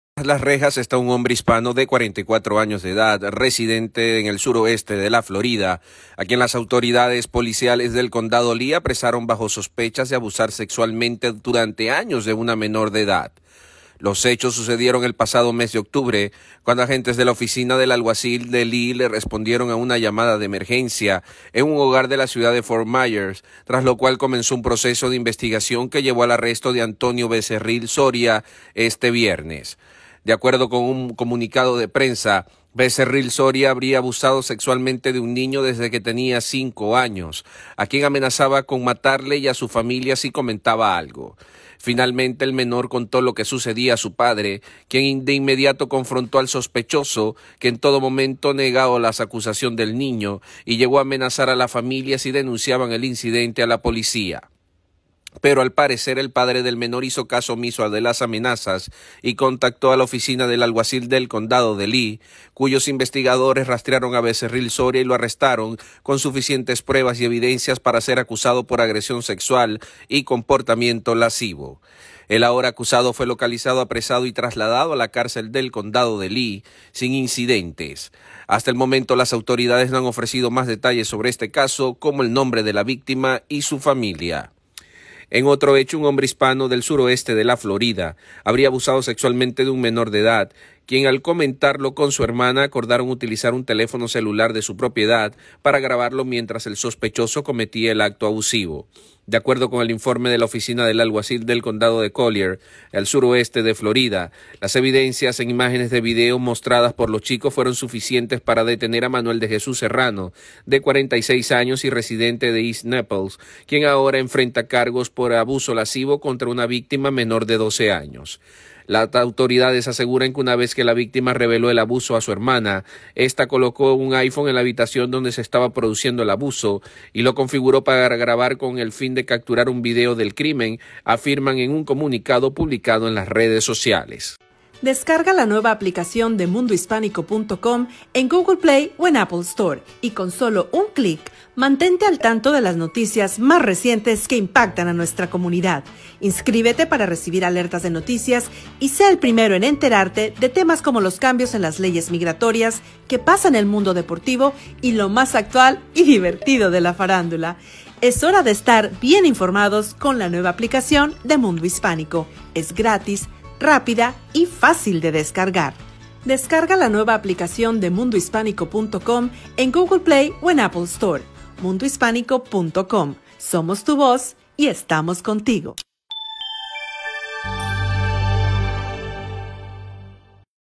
Hispano amenazó a su víctima durante años para que no lo delatara
Un menor de edad que fue abusado durante años por un hispano, conto la verdad para que el culpable de los hechos atroces este tras las rejas y es que el menor no podía contarlo antes porque al parecer estaba amenazado por el hombre de 44 años de edad.

Los hechos sucedieron el pasado mes de octubre, cuando agentes de la Oficina del Alguacil de Lee respondieron a una llamada de emergencia en un hogar de la ciudad de Fort Myers, tras lo cual comenzó un proceso de investigación que llevó al arresto de Antonio Becerril Soria este viernes.